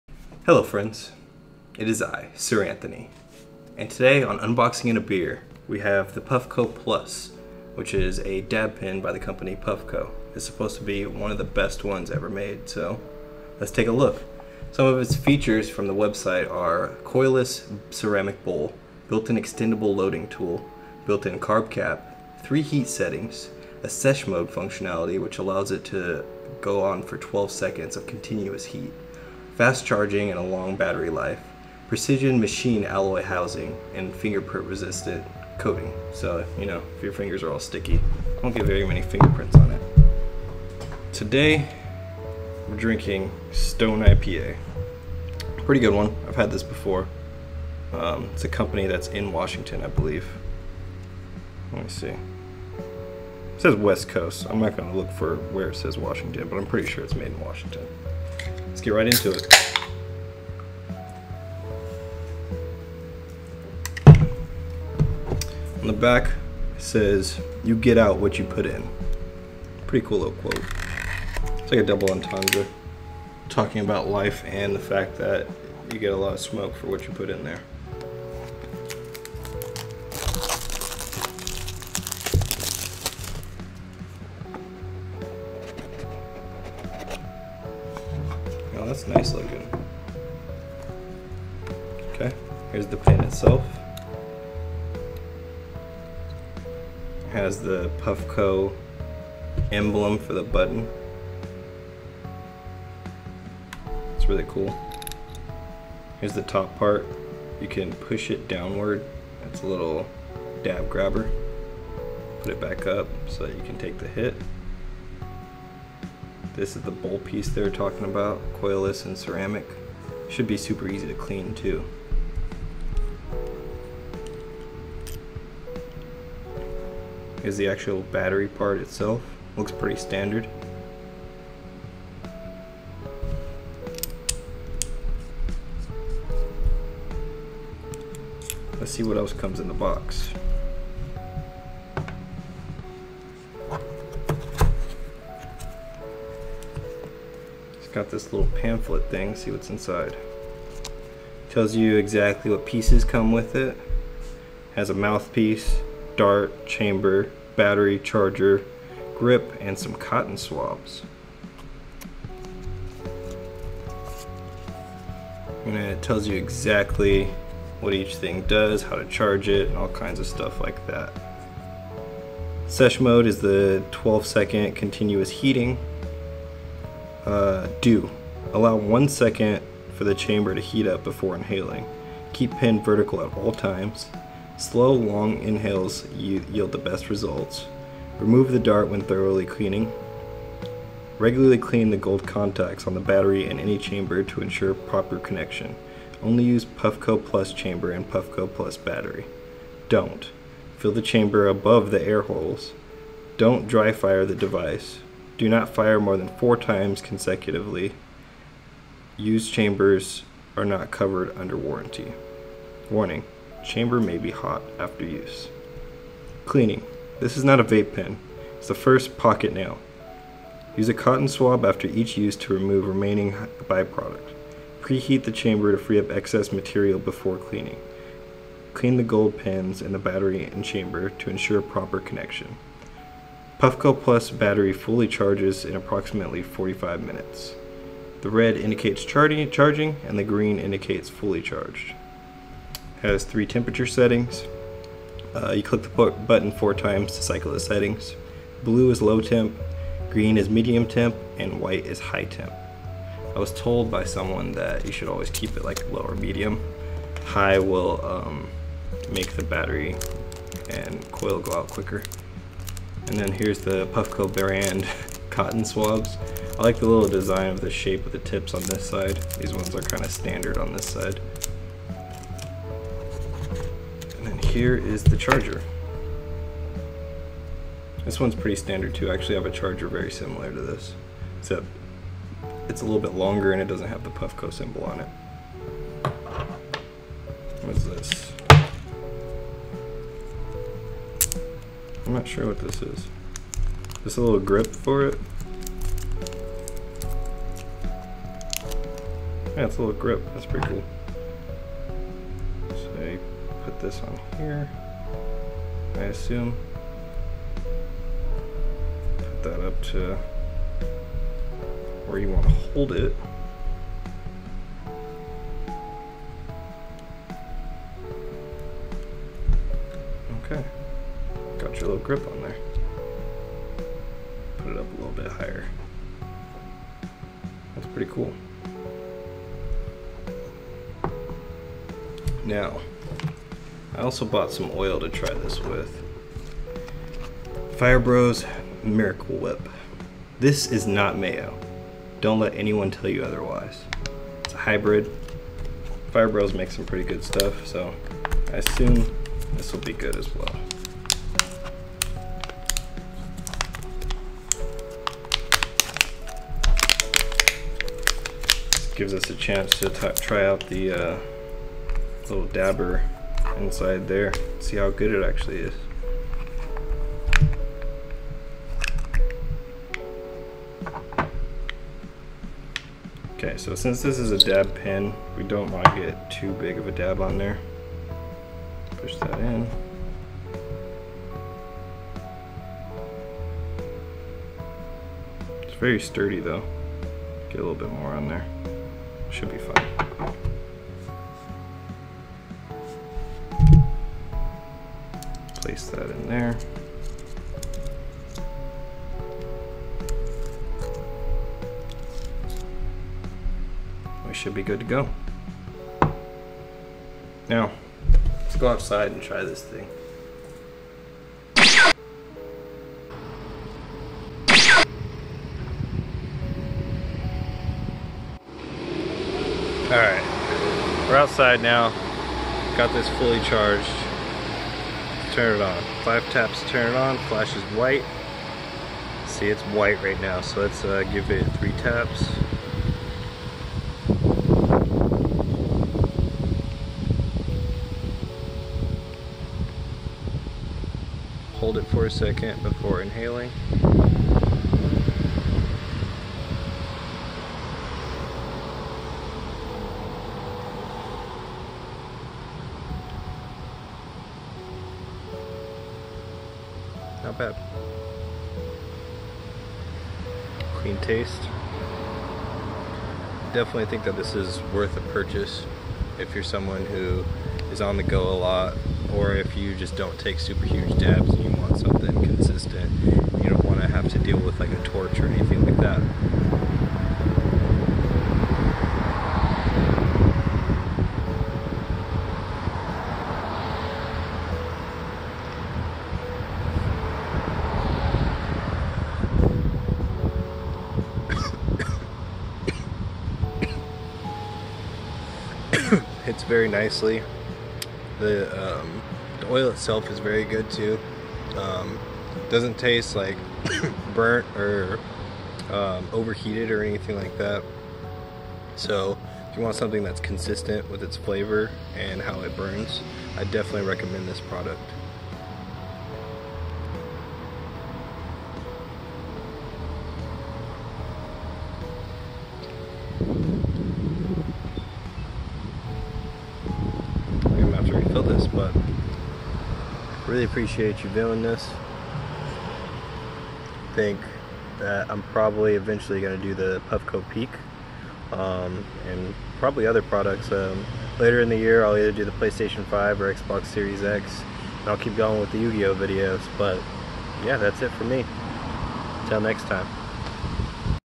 Unboxing and a Beer | Puffco Plus
In this video I take a look at the Puffco Plus and give my first impressions